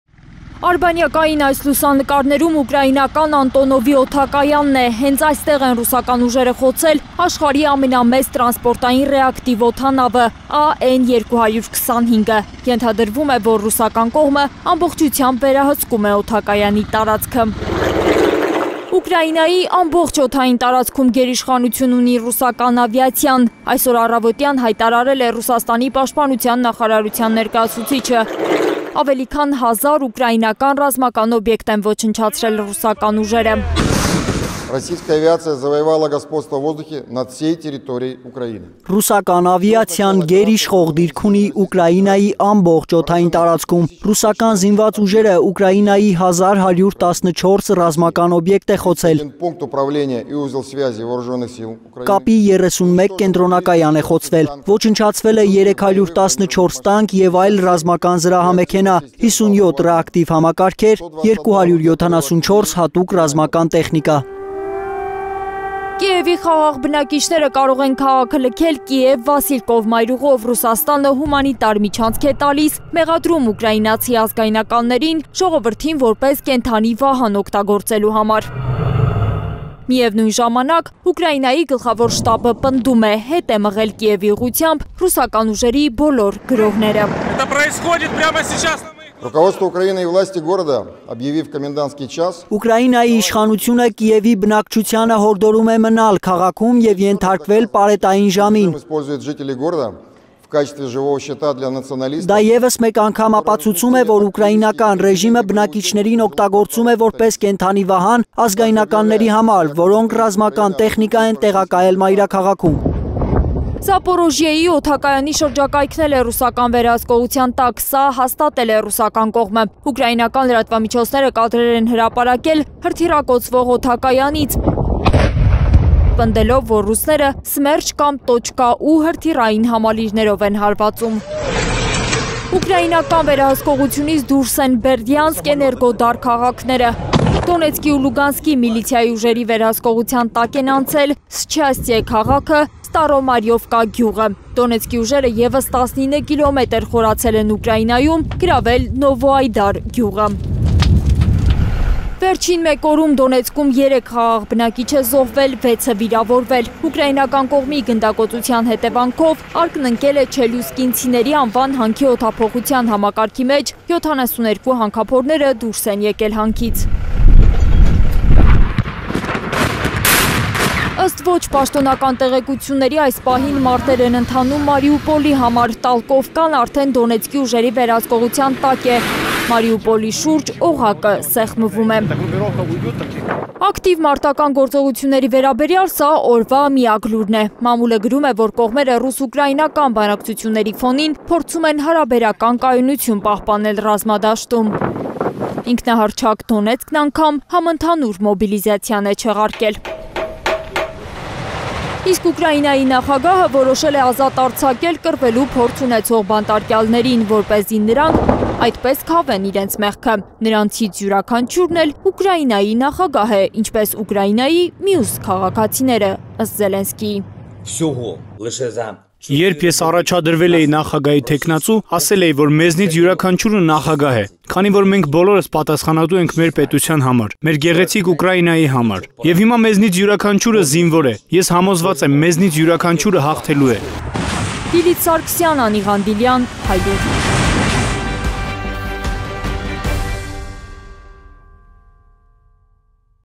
Զելենսկին պնդում է՝ «հաղթելու ենք».ռուսները գերիշխանություն ունեն Ուկրաինայի ամբողջ օդային տարածքում
Ռուսաստանը գերիշխում է Ուկրաինայի ամբողջ օդային տարածքում՝ վստահեցրել է Դաշնության պաշտպանության նախարարությունը և հայտնել, որ Կիևի բոլոր խաղաղ բնակիչները կարող են հանգիստ լքել Ուկրաինայի մայրաքաղաքը Կիև-Վասիլկով մայրուղով: